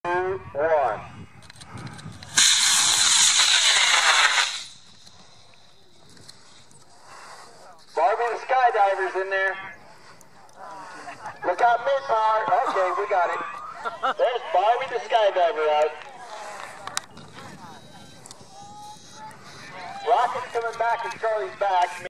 01_Freefall
My scratch built upscale Freefall XL on a G75 Blackjack motor. This time, with an astronaut Barbie skydiver. LUNAR Snow Ranch launch 040415.